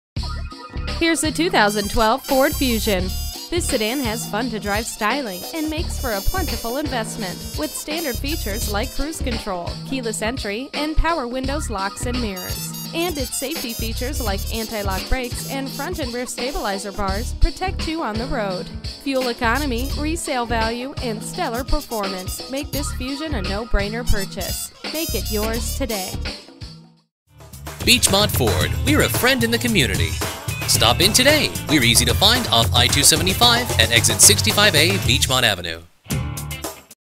2012 Ford Fusion C12-632 in Cincinnati Dayton, OH 45245 - SOLD
SOLD - If you are looking for real value on a great new car, Beechmont Ford invites you to come in and test drive this 2012 Ford Fusion, stock# C12-632. We are conveniently located near Cincinnati Dayton, OH and known for our great selection, reliability and quality. Come take a look at this 2012 Ford Fusion today.

Beechmont Ford
600 Ohio Pike
Cincinnati Dayton OH, 45245